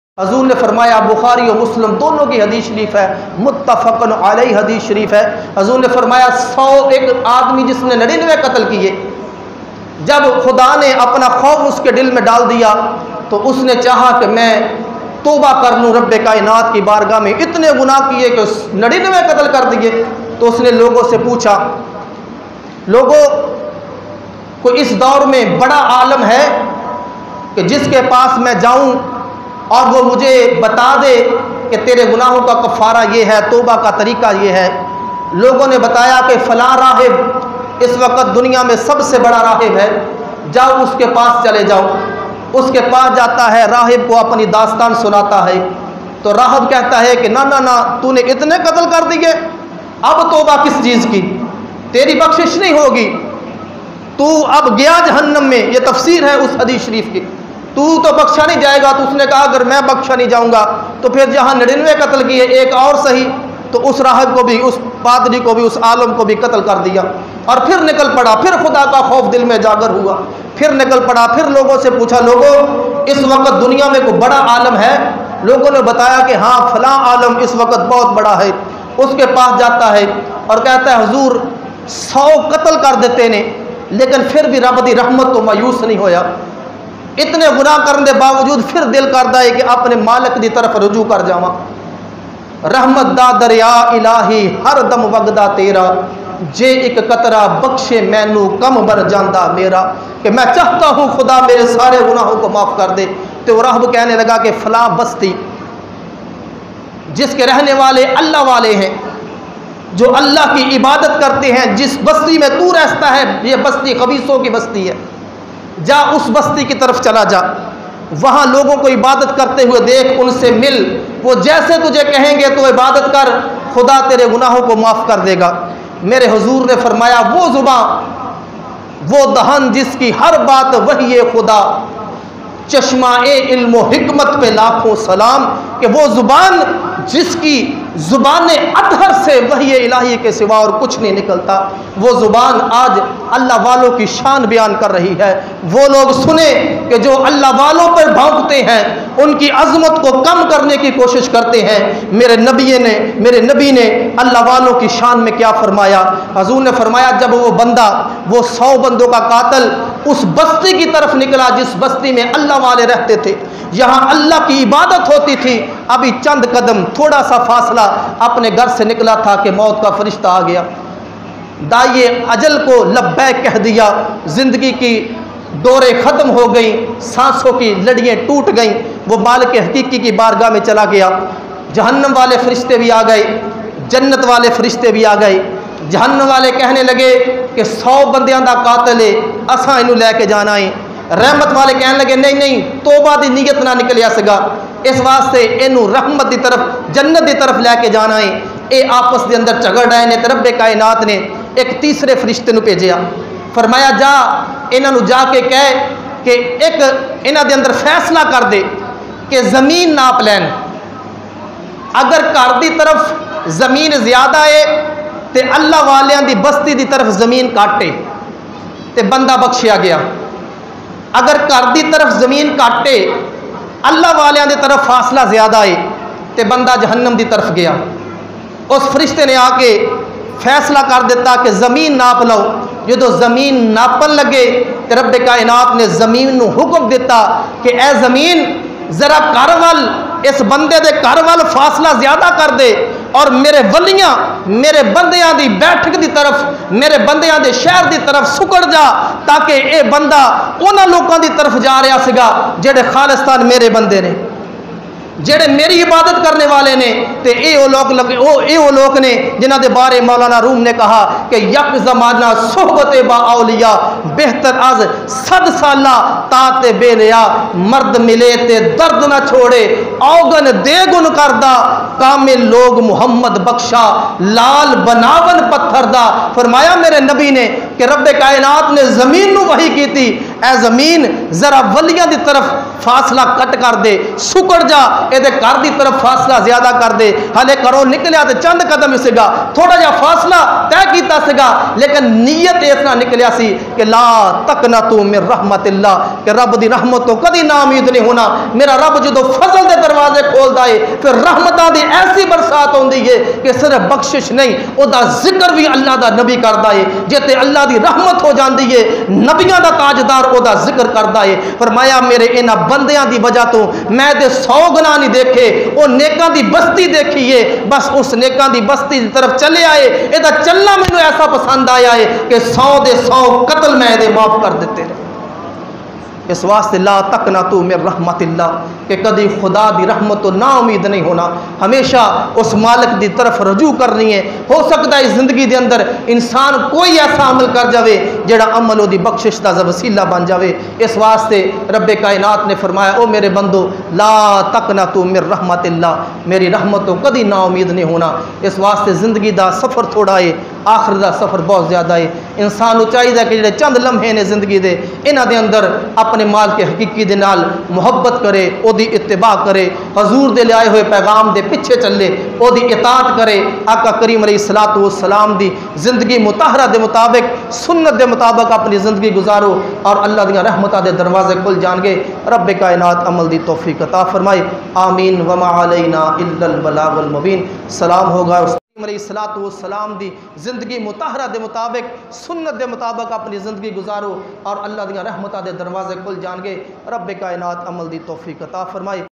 shan e Auliya Allah bazubane Mustafa by allama Muhammad Ismail qadri
شان اولیاء اللہ بزبان مصطفی صل علیہ وسلم سو بندوں کا قاتل بخشا گیا اللہ کی رحمت سے کبھی ناامید نہ ہونا بخاری شریف #